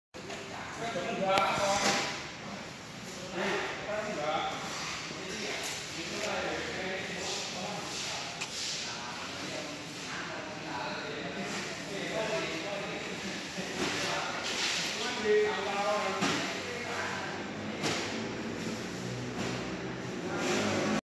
Drum plastik 200 L baru Dobel Ring sabuk. info hub WA : 08 123 266 850
Drum plastik 200 L baru dobel ring sabuk untuk pelampung kerambah ikan dll